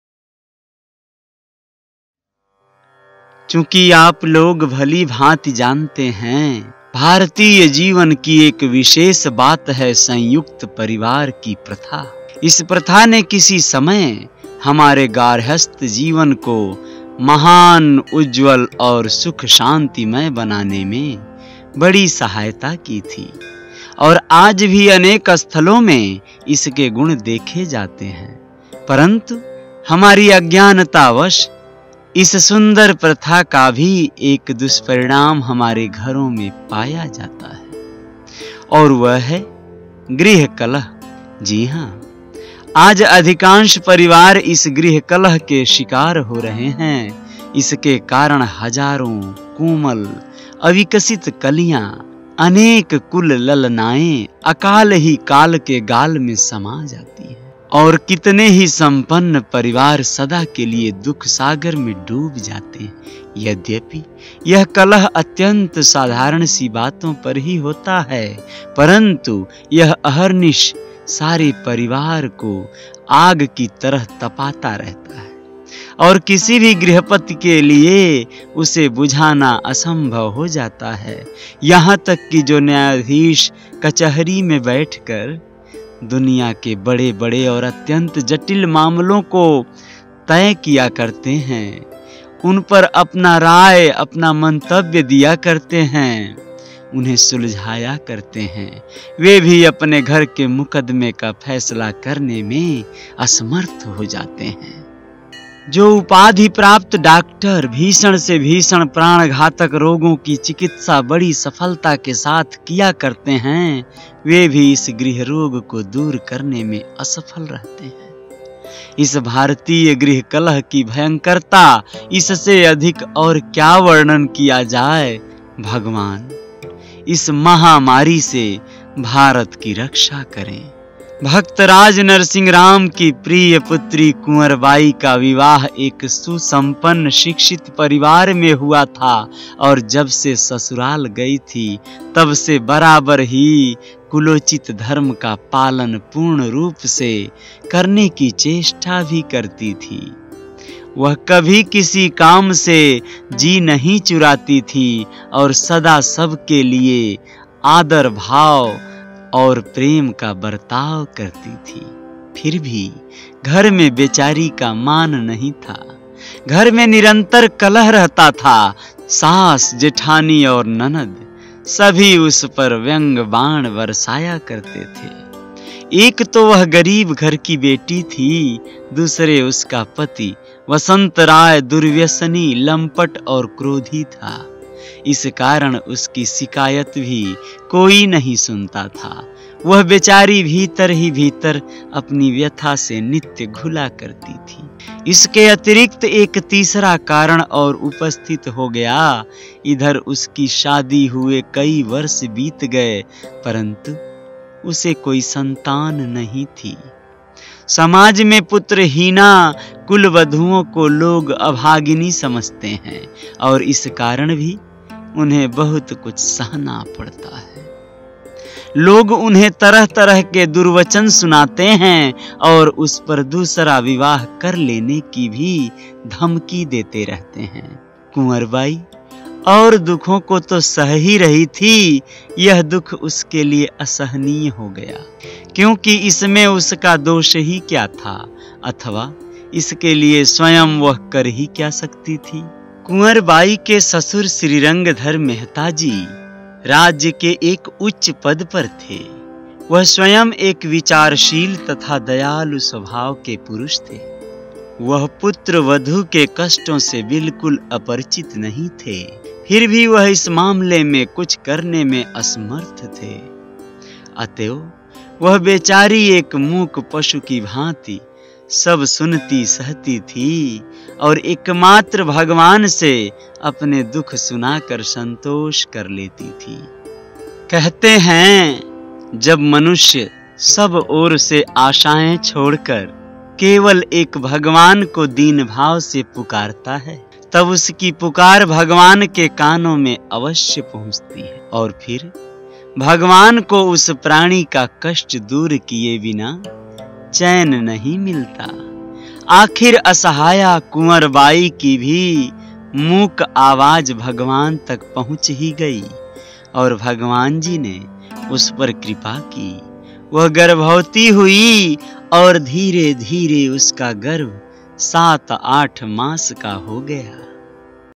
Narsinh Mehta Ki Khata | Audio Book | Part 31 | Narsinh Mehta Story in Hindi | Bhakt Narsinh Mehta
Presenting - Narasinh Mehta Ki Khata Speech By Ajay Sahani

Narsinh Mehta was born at Talaja and later moved to Junagadh (then Jirndurg) in Saurashtra, Gujarat. He lost his parents when he was 5 years old. He could not speak until the age of 8. He was raised by his grandmother Jaygauri.

He married Manekbai probably in the year 1429. Mehta and his wife stayed at his brother Bansidhar's house in Junagadh. However, his cousin's wife (Sister-in-law or bhabhi) did not welcome Narsinh very well. She was an ill-tempered woman, always taunting and insulting Narsinh Mehta for his worship (Bhakti). One day, when Narasinh Mehta had enough of these taunts and insults, he left the house and went to a nearby forest in search of some peace, where he fasted and meditated for seven days by a secluded Shiva lingam until Shiva appeared before him in person. On the poet's request, the Lord took him to Vrindavan and showed him the eternal raas leela of Krishna and the gopis. A legend has it that the poet, transfixed by the spectacle, burnt his hand with the torch he was holding, but he was so engrossed in the ecstatic vision that he was oblivious to the pain. Mehta, as the popular account goes, at Krishna's command, decided to sing His praises and the nectarous experience of the rasa in this mortal world. He resolved to compose around 22,000 kirtans or compositions.